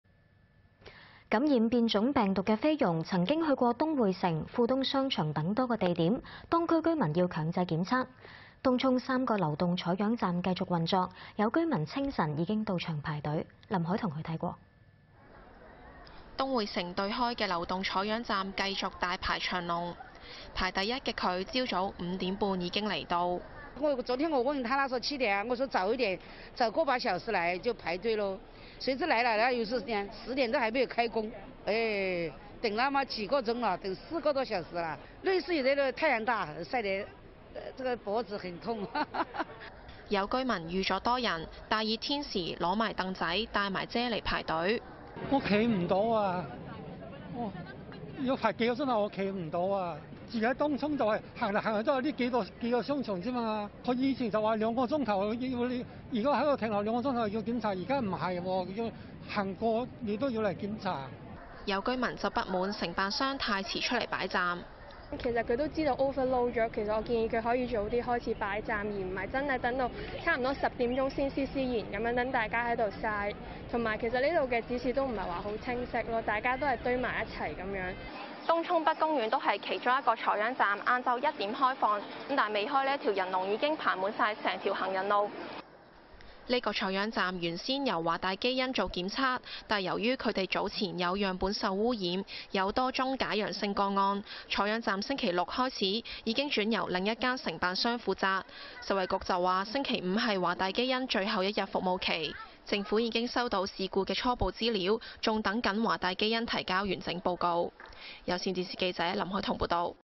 有承辦商疑遲設採樣站 有東涌居民候逾四小時強檢、批指示不清晰 - 20210501 - 港聞 - 有線新聞 CABLE News
【有線新聞】感染變種病毒的菲傭曾到東薈城、富東商場等地，政府限令當區居民強制檢測。東涌三個流動採樣站繼續運作，有居民清晨已到場排隊。東薈城對開的流動採樣站繼續大排長龍，排首位的郁太上午五時半已經來到，「昨天我問他（工作人員），他說七時開始（檢測），我想提早一個半小時，誰知來了又是這樣，十時仍未開工。等那麽久、數個小時，等四個多小時了。累是累，太陽大，曬得脖子很痛。」有居民已預計人數眾多，大熱天時準備椅子、雨傘來排隊。張先生：「我站不到，排了數小時。居住東涌逛來逛去也是數個商場，以前逗留兩小時要檢測，現在不是，經過也要檢測。」有居民不滿承辦商太遲設採樣站。莊小姐：「其實他都知道人數會過多，建議他們早點擺站，不是差不多十時才慢慢地設站，讓大家曬。這裏的指示不清晰，大家堆在一起。」另一個採樣站東涌北公園下午一時才開放。惟仍未開放，人龍已站滿整條行人路。這個採樣站原本由華大基因做檢測，但由於較早前有樣本受污染致有多宗假陽性個案，故周六開始已轉由另一間承辦商負責。食衞局指周五是華大基因最後一天服務期，當局已收到事故初步資料，仍待華大基因提交完整報告。

相關影片:
1. DSE中文科閱讀卷首用日本作家翻譯作　考生：不清楚背景難明白感情 - 20210429 - 港聞 - 有線新聞 CABLE News
3. 紅火蟻已擴散至中國12個省份 - 20210428 - 有線中國組 - 有線新聞 CABLE News